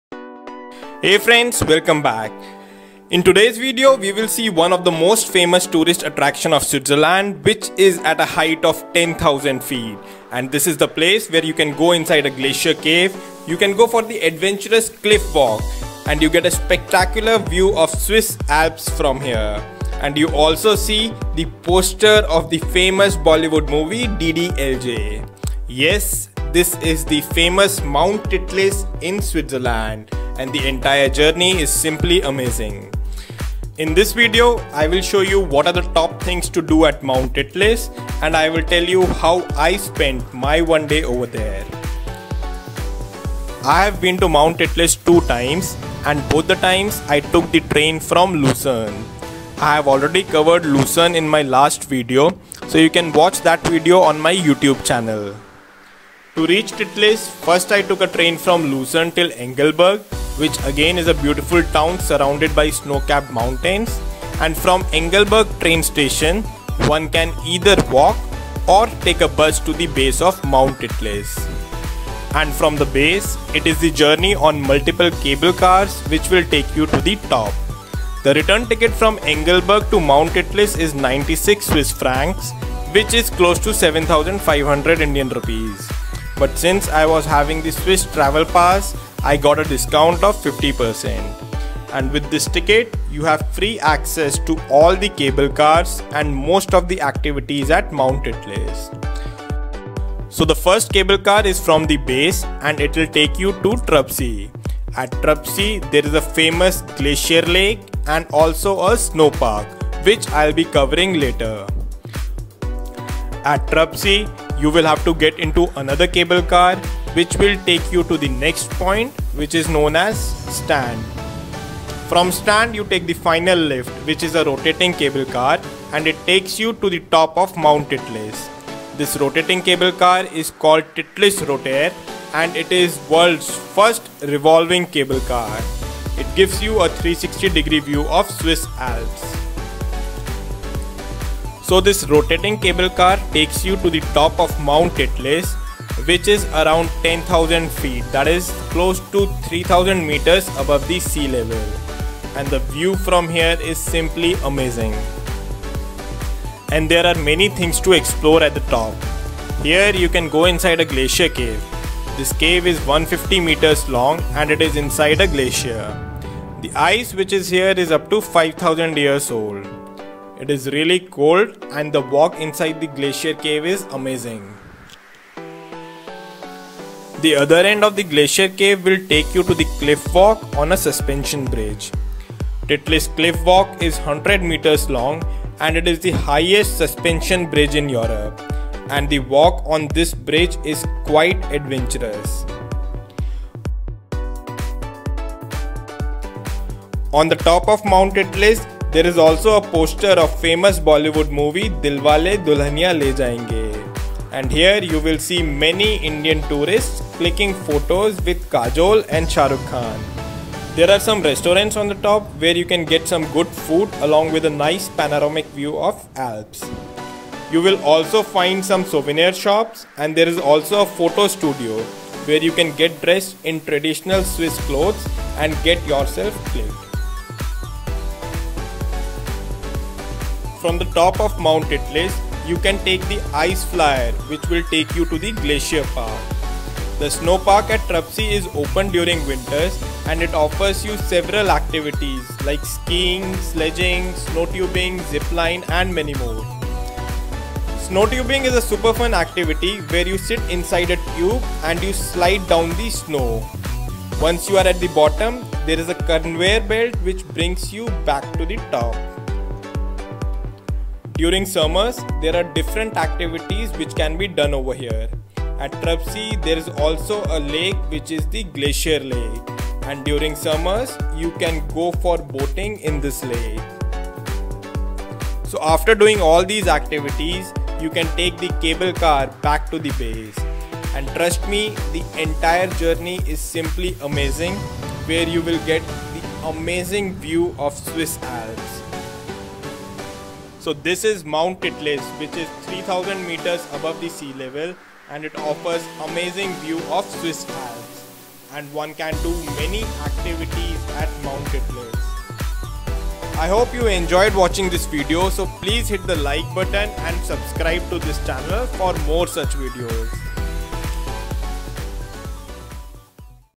Mount Titlis SWITZERLAND | Things to Do at Mt. Titlis Switzerland - Take the Titlis Cable Car
Mt Titlis is one of the Most famous tourist attraction of Switzerland. In this video we will see the top things to do at Mt Titlis Switzerland.
Below is a brief summary of things covered in this video:
To reach Mt Titlis one can take a train from Luzern to Engelberg. And from From Engelberg one can take Titlis Cable Car to reach to the top of Mt Titlis. The 1st Cable Car takes you from Engelberg to Trubsee. From Trubsee take another cable car to reach Stand. And from Stand take Titlis Rotair (a rotating cable car) to reach the top of Mt Titlis. Titlis Rotair is world's 1st revolving cable car which gives 360 degree view of Swiss Alps. Mt Titlis is 10000 Feet / 3020 Meters above sea level. One can do several activities at Mt Titlis.
Below is a list of Things that one can to Do at Mount Titlis:
1) Take the Titlis Glacier Walk
2) Walk on Titlis Cliff Walk - the highest Suspension Bridge of Europe
3) Take Photo with Poster of Bollywood Movie DDLJ
4) Eat some nice food at Titlis Restauran
5) Buy Mt Titlis Souvenir
6) Get dressed in traditional Swiss Clothes and get clicked
7) Take Ice Flyer from Mt Titlis to Glacier Park
8) Visit Snow park in Trubsee (Winters)
9) Snowtubing at Trubsee Snow Park (Winters)
10) Boating in Lake Trubsee in Summer
11) And enjoy the beautiful view of Alps and do many other things :)
OTHER VIDEOS ON EUROPEAN CITIES ON THIS CHANNEL
OTHER VIDEO PLAYLIST ON THIS CHANNEL